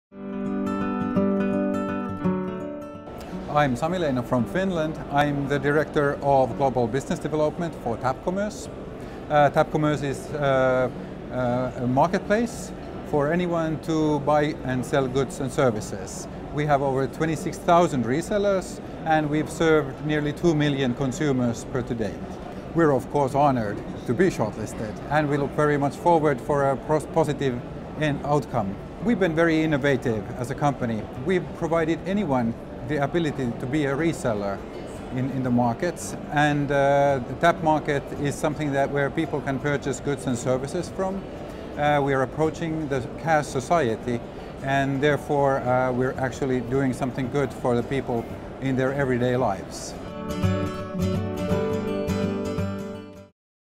Best New E-Commerce Merchant - Tapp Market
SMART Awards Asia 2016 WINNER
Quick chat with Sami Leino, Global Business Development Director of Tapp Market